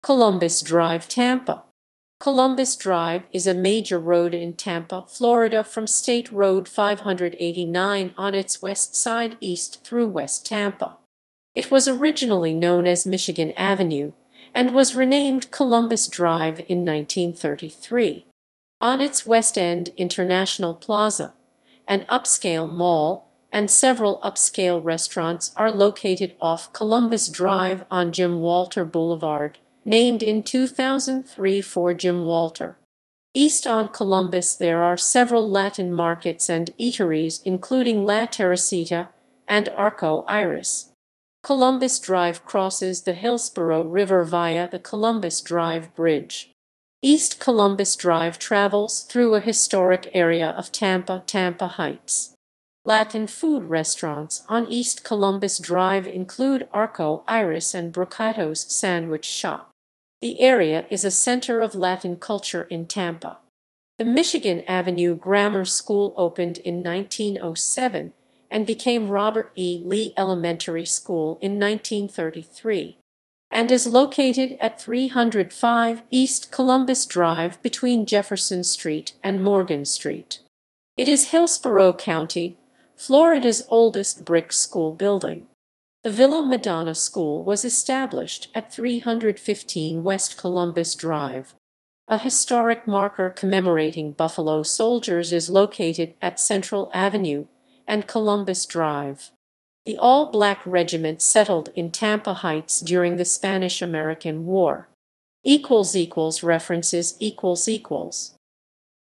Columbus Drive (Tampa)
Columbus Drive is a major road in Tampa, Florida from State Road 589  on its west side east through West Tampa. It was originally known as Michigan Avenue and was renamed Columbus Drive in 1933. On its west end, International Plaza, an upscale mall, and several upscale restaurants are located off Columbus Drive on Jim Walter Boulevard, named in 2003 for Jim Walter. East on Columbus there are several Latin markets and eateries including La Teresita and Arco Iris. Columbus Drive crosses the Hillsborough River via the Columbus Drive Bridge.East Columbus Drive travels through a historic area of Tampa, Tampa Heights. Latin food restaurants on East Columbus Drive include Arco-Iris and Brocato's Sandwich Shop. The area is a center of Latin culture in Tampa. The Michigan Avenue grammar school opened in 1907 and became Robert E. Lee Elementary School in 1933. and is located at 305 East Columbus Drive  between Jefferson Street and Morgan Street. It is Hillsborough County, Florida's oldest brick school building. The Villa Madonna School was established at 315 West Columbus Drive.A historic marker commemorating "Buffalo Soldiers" is located at Central Avenue and Columbus Drive. The all-black regiment settled in Tampa Heights during the Spanish–American War.

== References ==